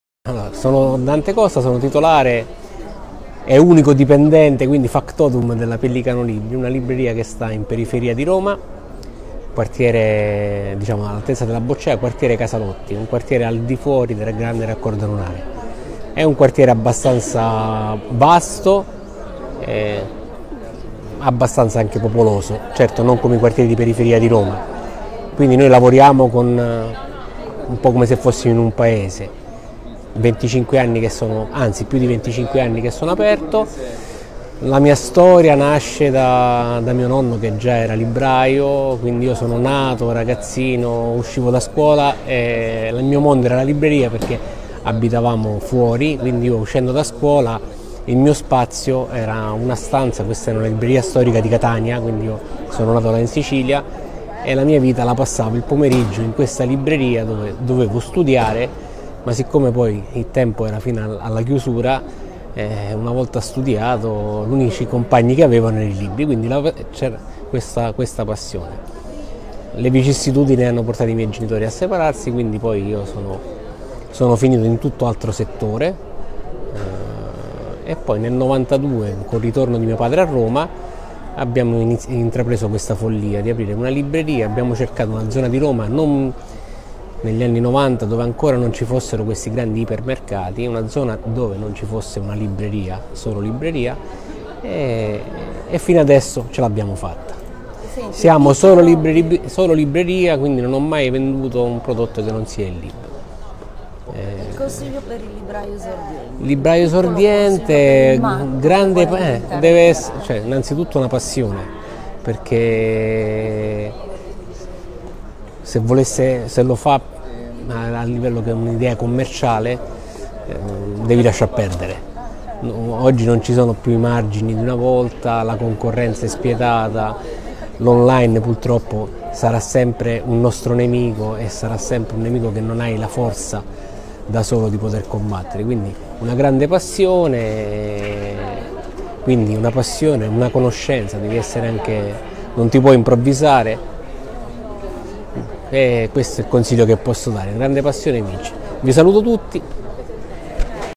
STORIE DI LIBRAI, Dante Costa Pellicanolibri, Roma
STORIE DI LIBRAI
Dante, della Pellicano Libri, libreria nella periferia romana, quartiere Casalotti, da bambino usciva da scuola ed entrava nella libreria del nonno. E, a quel punto, i compagni diventavano proprio loro, i libri… Il suo consiglio per il libraio esordiente? Una sufficiente dose di passione…
video realizzato da Festbook